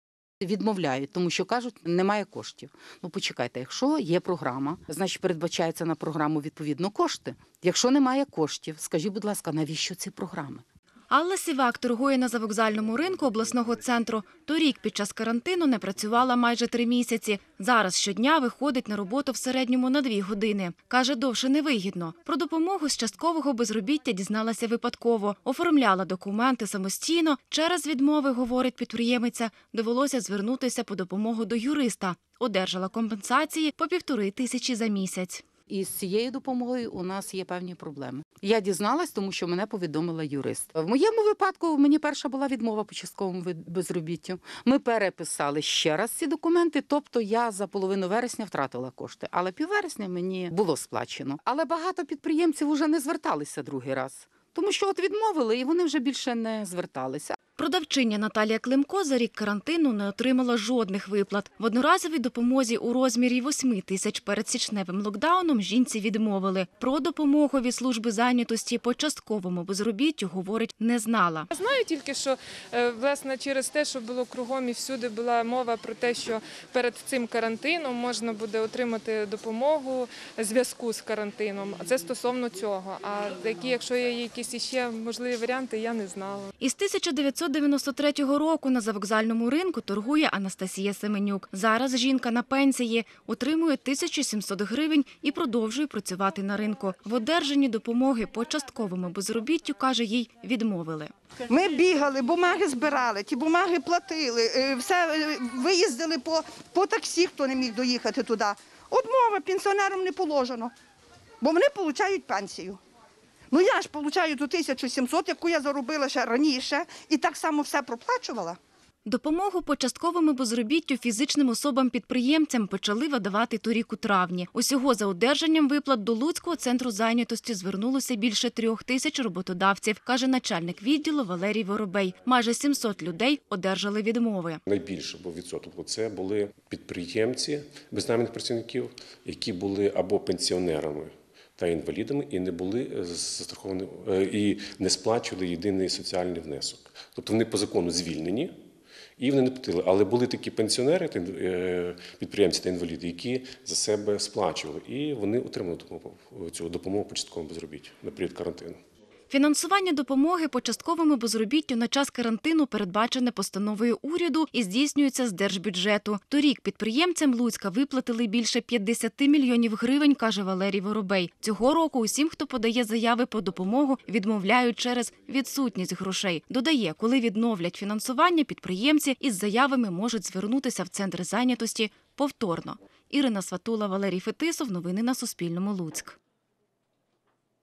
Чому підприємці на Волині не отримують допомогу по безробіттю через карантин
Із початку року волинським підприємцям не виплачують допомоги по частковому безробіттю на період карантину. Тим, хто звертається до центрів зайнятості із відповідними заявами, надсилають відмови.